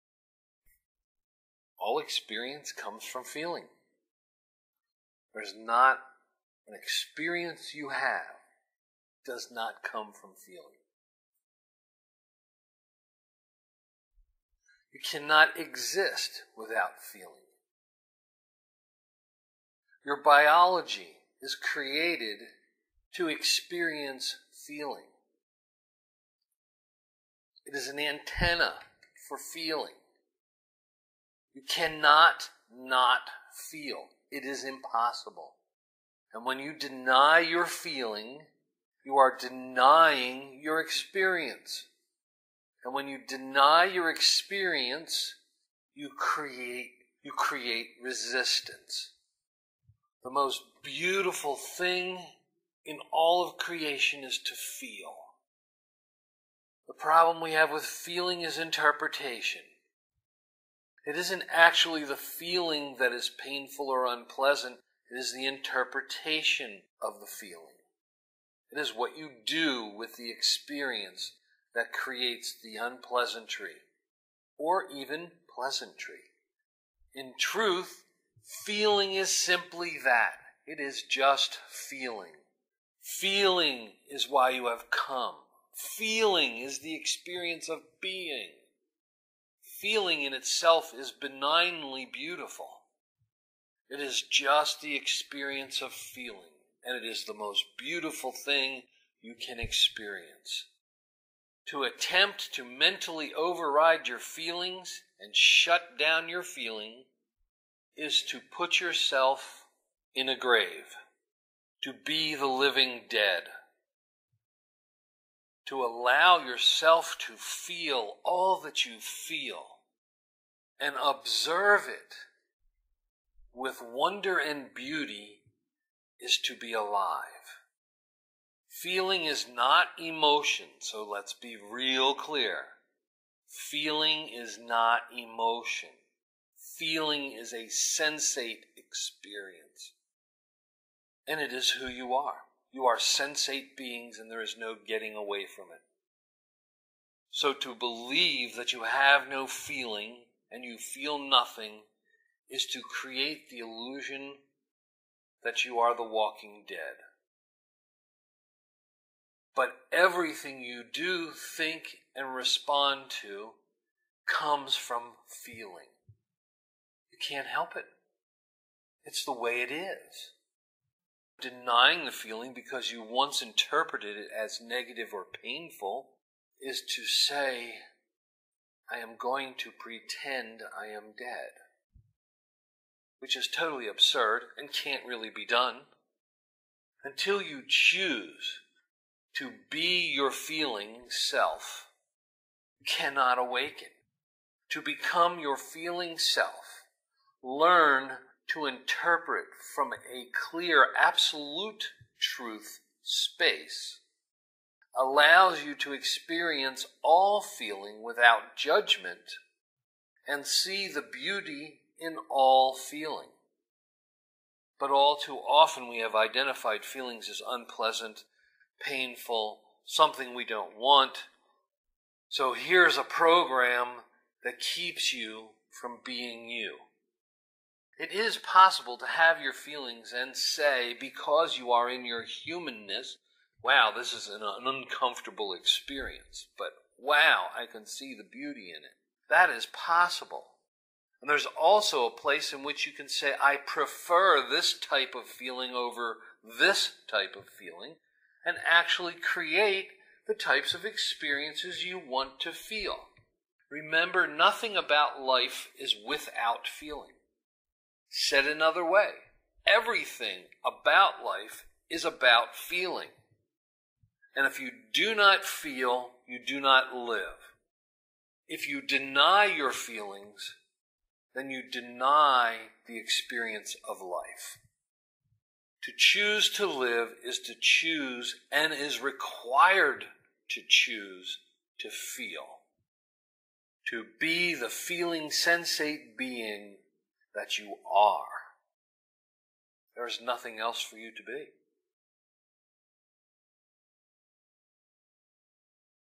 FEELING {by Michael Cavallaro}
All experience comes from FEELING. You cannot exist without feeling. Be the CREATOR of your life...feel and BE all your experiences. The most beautiful thing in life is feeling. To allow yourself to feel, all that you feel, and observe with wonder and beauty, is to be alive! Feeling is a sensate experience and feeling is who you are as a sensate being.

Read Michael's acclaimed self-help book "The 55 Concepts: A Guide to Conscious Living" TODAY